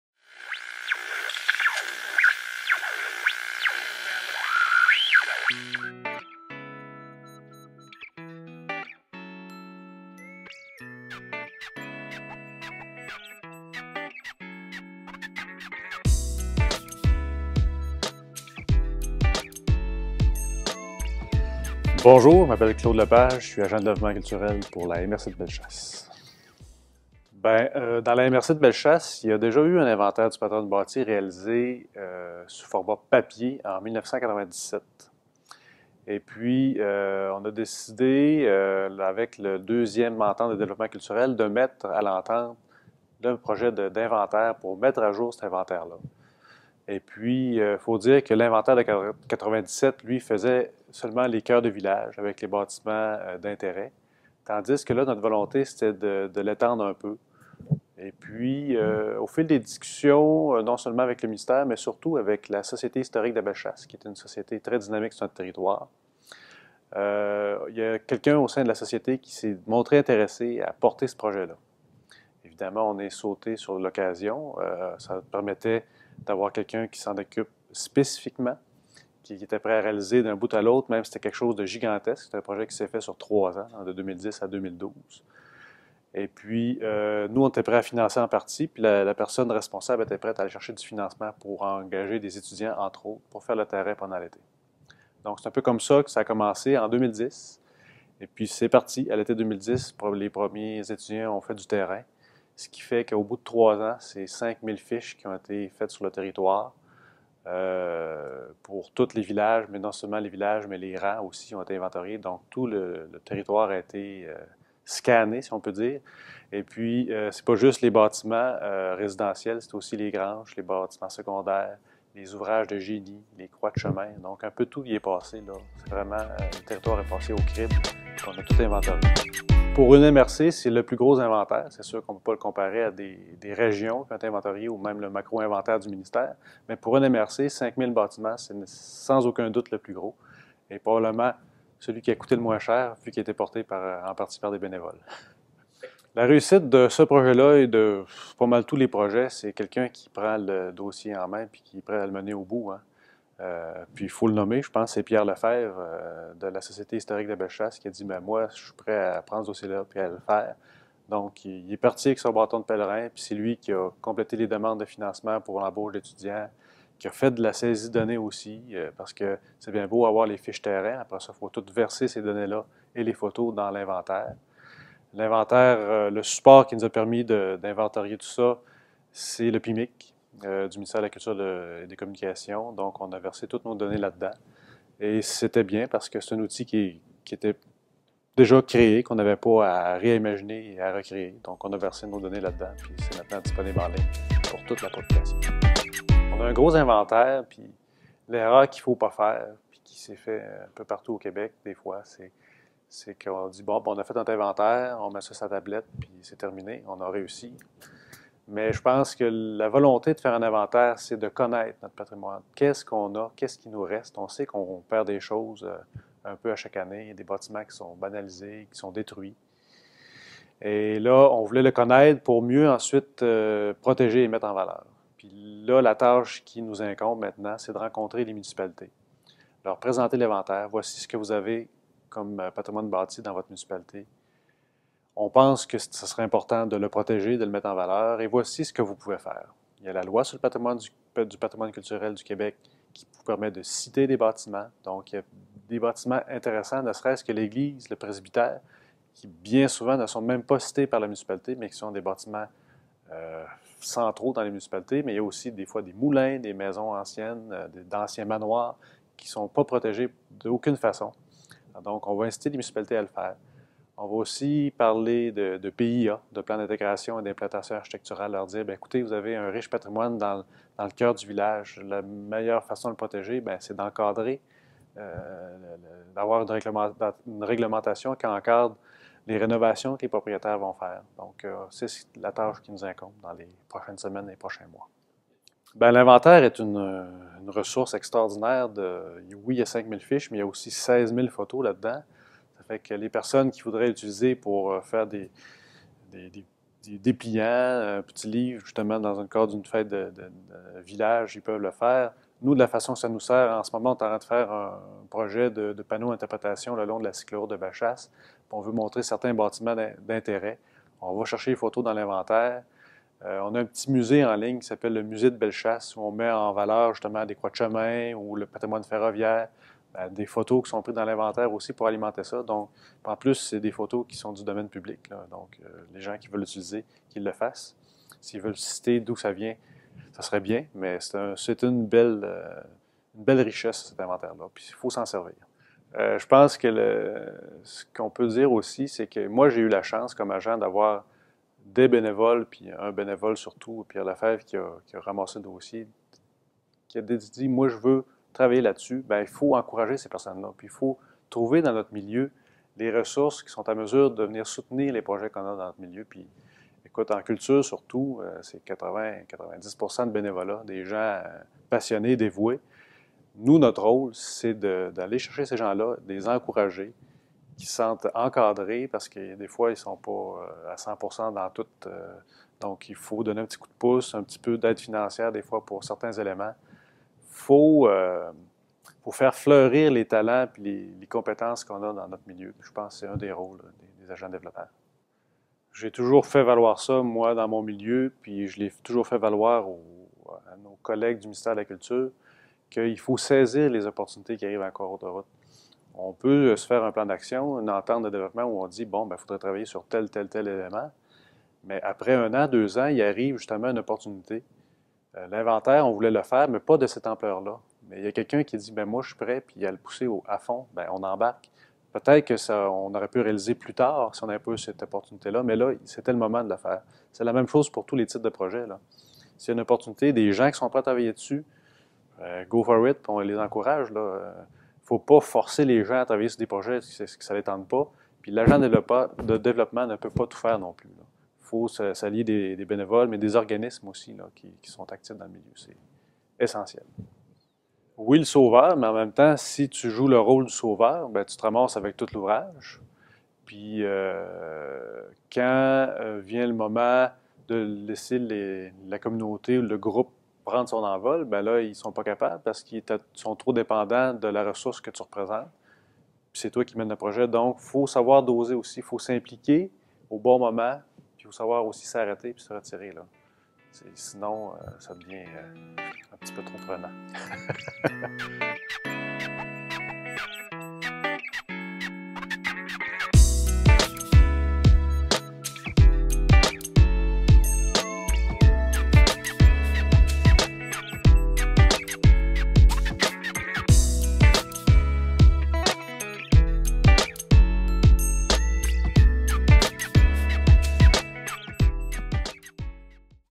Inventaire du patrimoine bâti, MRC de Bellechasse
Claude Lepage, agent de développement culturel, présente le projet d'inventaire du patrimoine bâti réalisé dans la MRC de Bellechasse.